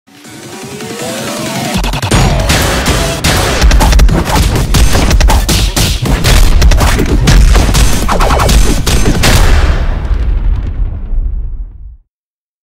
Gunner Vs Speed (hatena's soundline)
This is a little soundline test I wanted to try
Animation from: Mek
Soundline from: Hatena